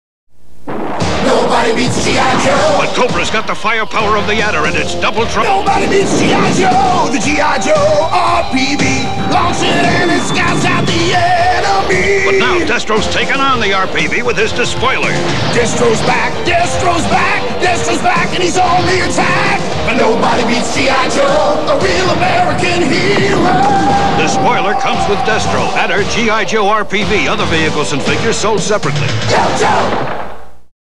GIJoe 1988 TV Commercial 05: RPV/Despoiler/Twin Missile - from Griffin Bacal Inc VHS Master
Griffin Bacal Inc. was a global advertising agency founded in 1978 by Tom Griffin and Joe Bacal. Hasbro partnered with Griffin Bacal to create advertising campaigns for G.I.Joe: A Real American Hero, Transformers, and more. This video was digitized from a Griffin Bacal Inc. VHS master dated 3/2/88 and labeled "G.I.Joe 1988 Campaign."